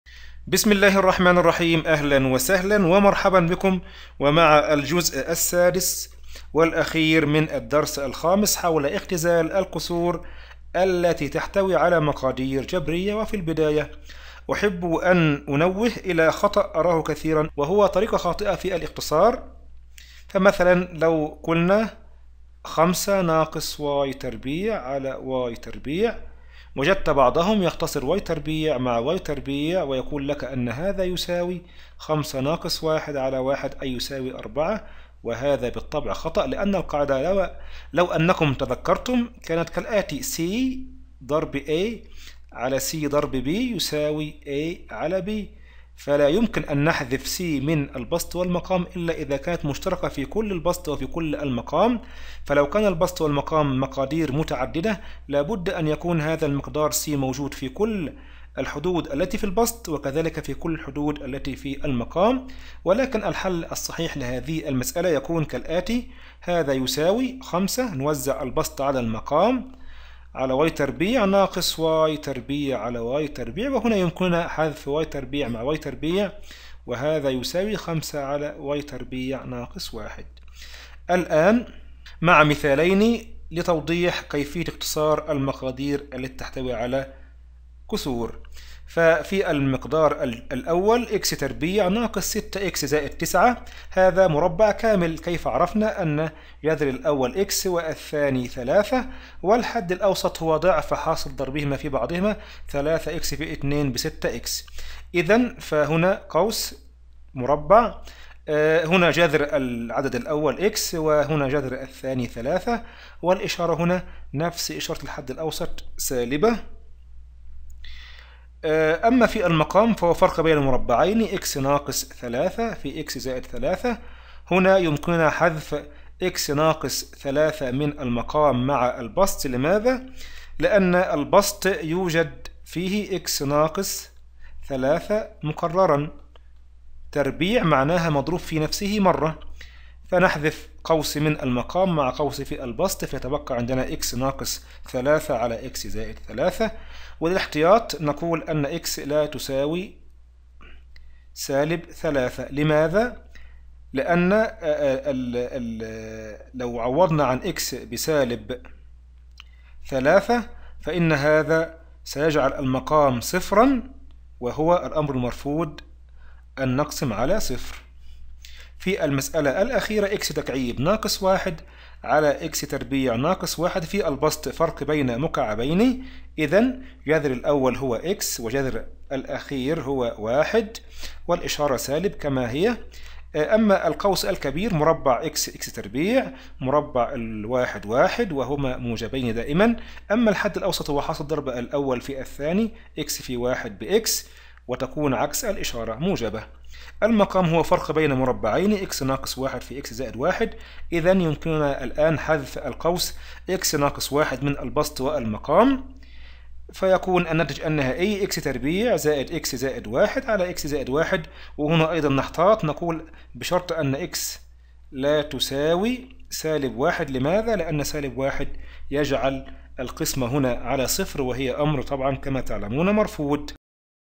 مهارات في علم الرياضيات - اختزال الكسور التي تحتوي على مقادير جبرية
المقررات المفتوحة
المهارات الأساسية في الحسابات الرياضية
Basic Skills in Mathematical computations
كثيرات الحدود
Polynomials
اختزال الكسور التي تحتوي على مقادير جبرية.
Reducing rational Expressions
1- التعرف على بعض طرق اختزال الكسور ذات المقادير الجبرية.
2- أمثلة توضيحية.
1. Reducing rational Expressions.
2. Examples.